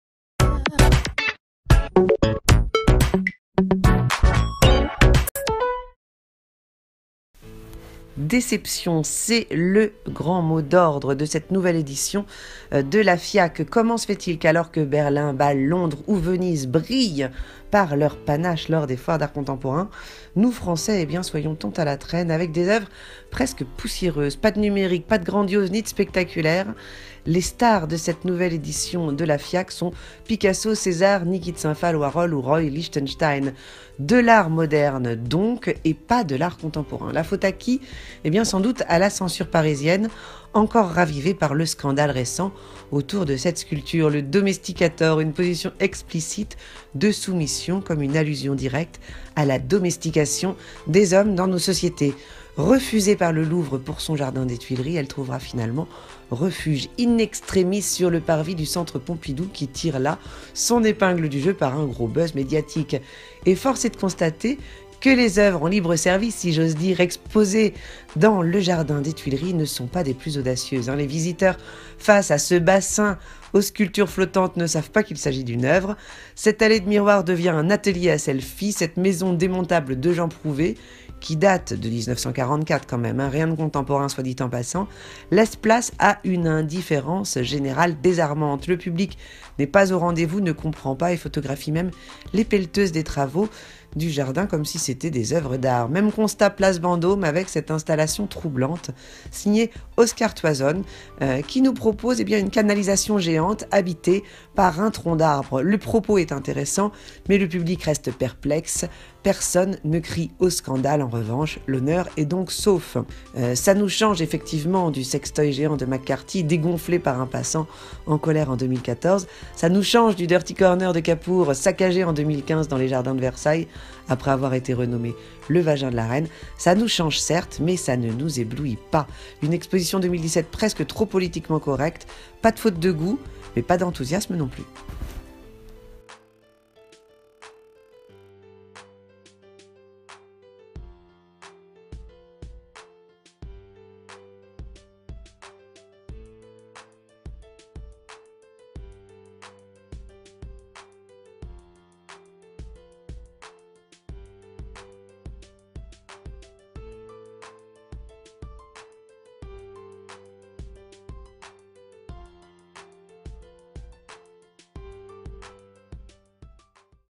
La Fiac est-elle devenue le temple de la censure et du politiquement correct ? Vidéo YouTube
Que penser d'une foire d'art contemporain internationale qui bride ses artistes, les censure et les encadre à outrance... Oeuvres écologiques, trop bien pensantes et attendues ? Pourquoi cette nouvelle édition de la FIAC ne reçoit-elle que si peu d'enthousiasme du public ?